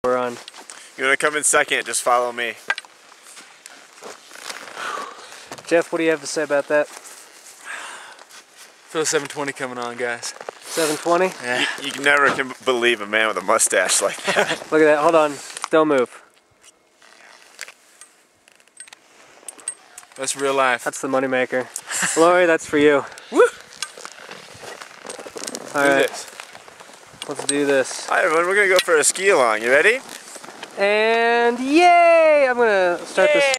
Jeff's Mustache looks good!
We talk and Jeff has a sweet Mustache!